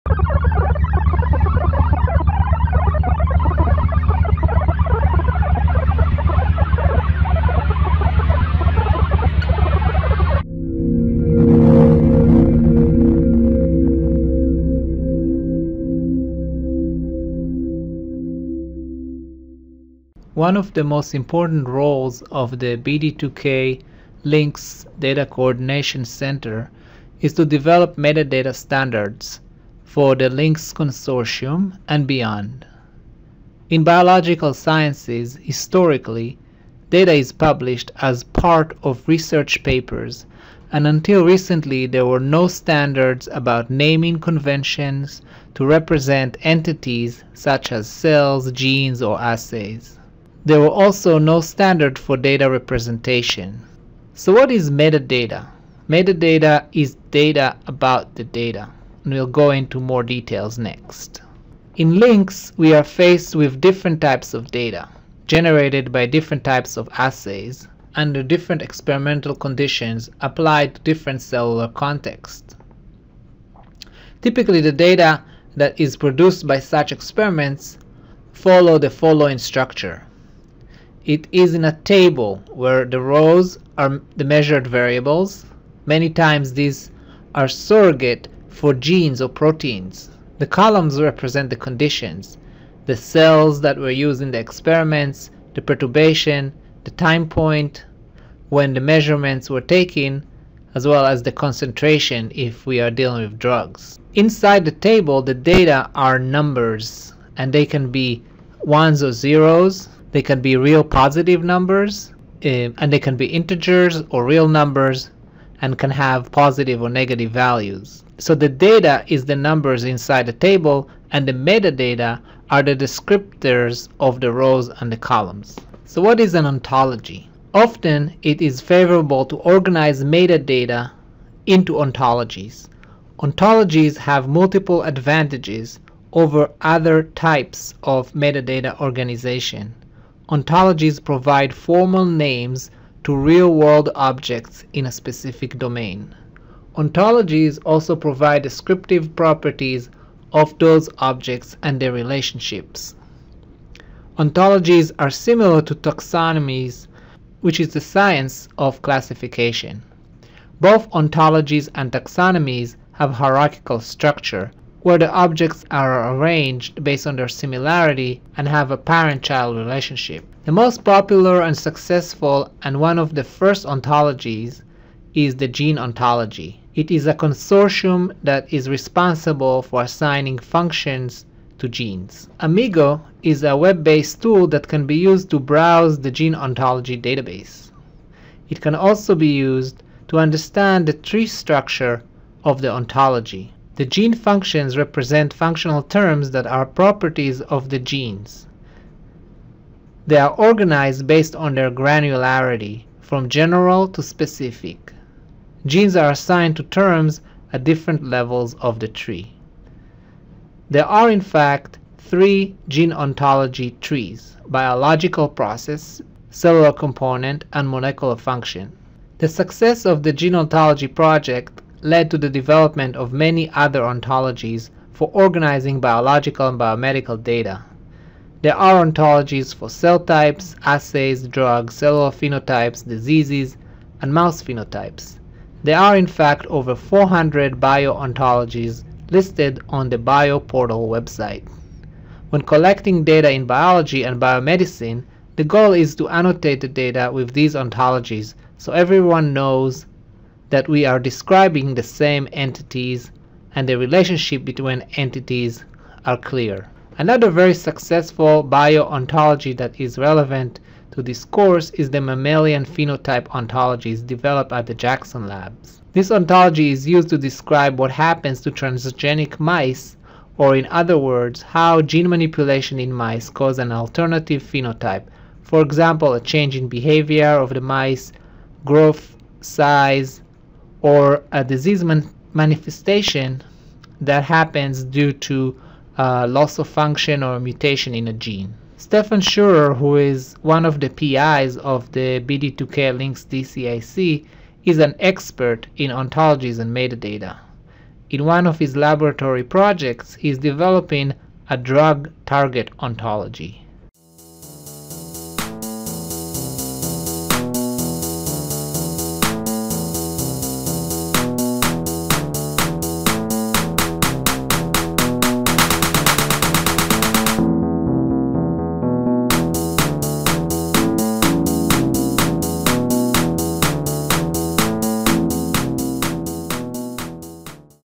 Lecture 9 - Metadata and Ontologies
In this lecture Professor Ma'ayan briefly describes basic concepts about metadata and ontologies.